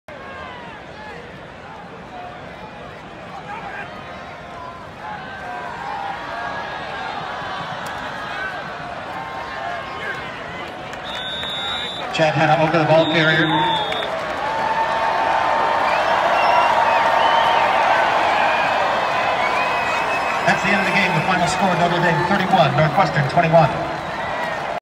Final Play of ND vs. Northwestern
November 3, 2018. Notre Dame 31 - Northwestern 21